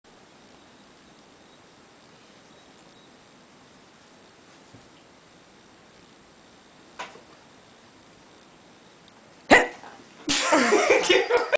Branca playing with daddy!!!
Branca doesn't like that you look straight at her ... if you dare, she attacks!
For some people who think this is a threat: RELAX! We are just playing. Branca will NEVER attack us